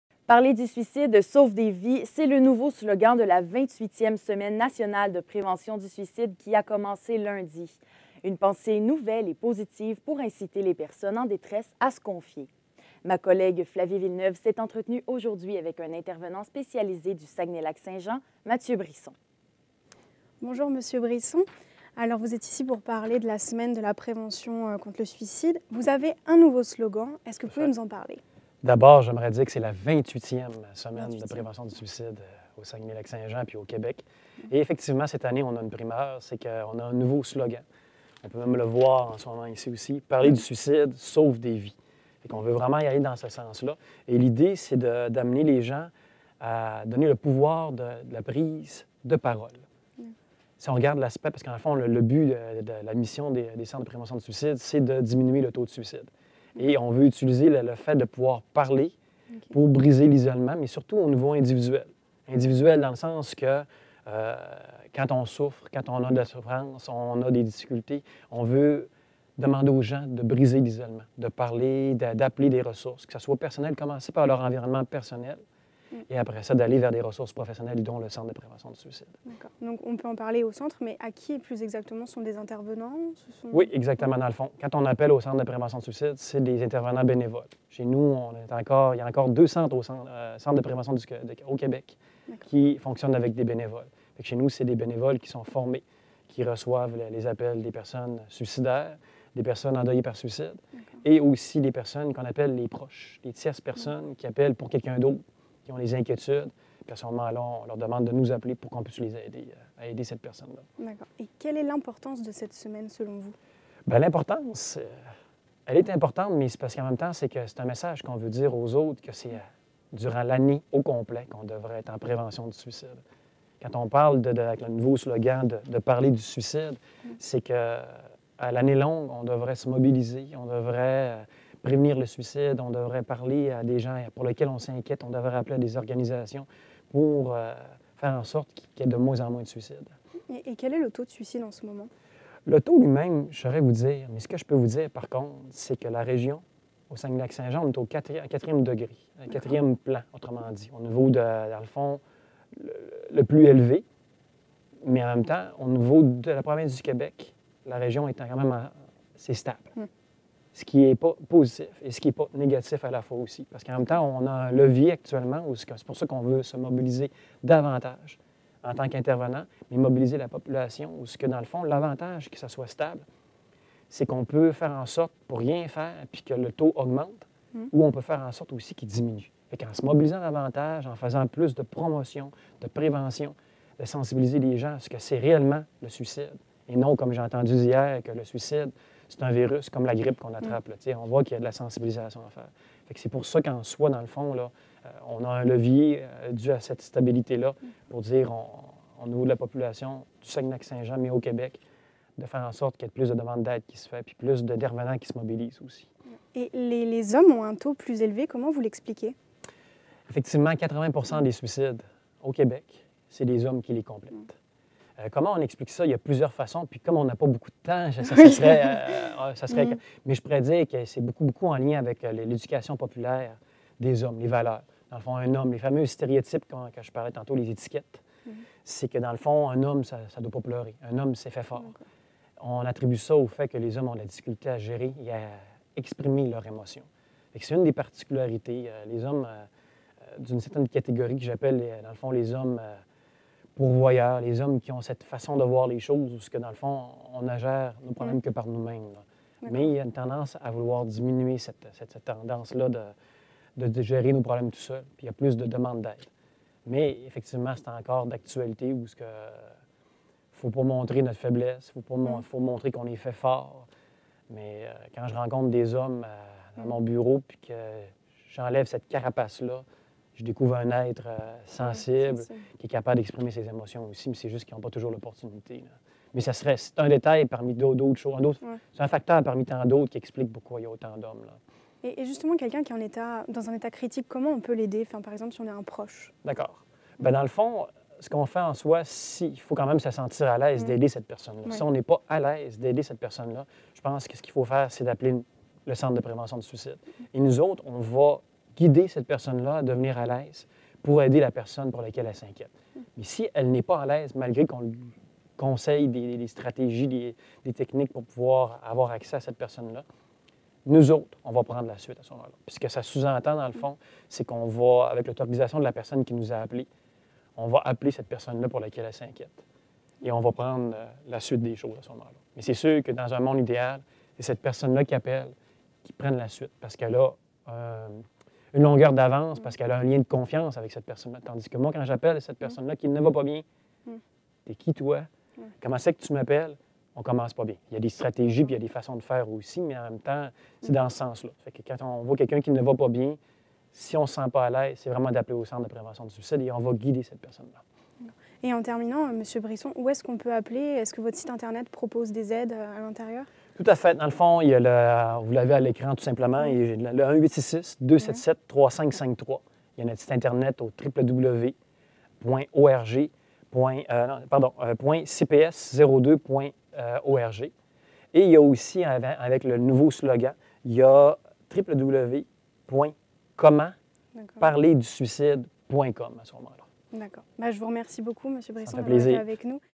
Mathieu Brisson Session hiver 2018
«Parler du suicide sauve des vies», c’est le nouveau slogan de la 28e semaine nationale de prévention du suicide qui a commencée lundi.

Une pensée nouvelle et positive pour inciter les personnes en détresse à se confier.

 Ma collègue Flavie Villeneuve s’est entretenue aujourd’hui avec un intervenant spécialisé du Saguenay-Lac-Saint-Jean, Mathieu Brisson.

Simone Pilote à la recherche.